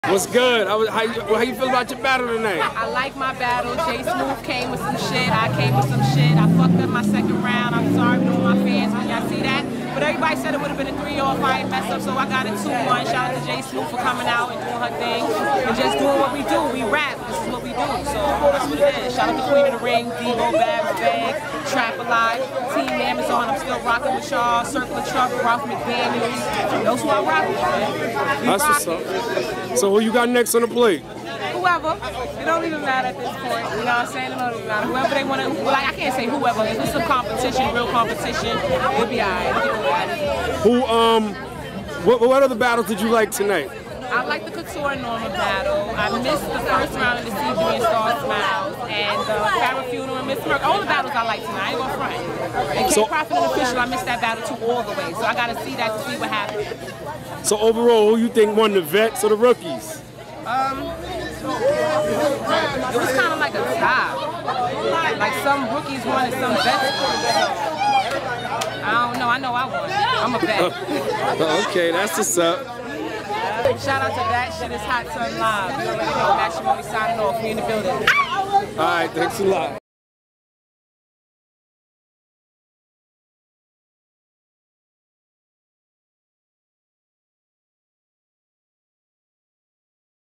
Queen Of The Ring "No Backing Out" Recap Matrimony talks about her battle with Jai Smooth
Queen Of The Ring "No Backing Out" Matrimony talks about her and Jai Smooths battle and the overall outcome between the vets and the rookies go for all battles and interviews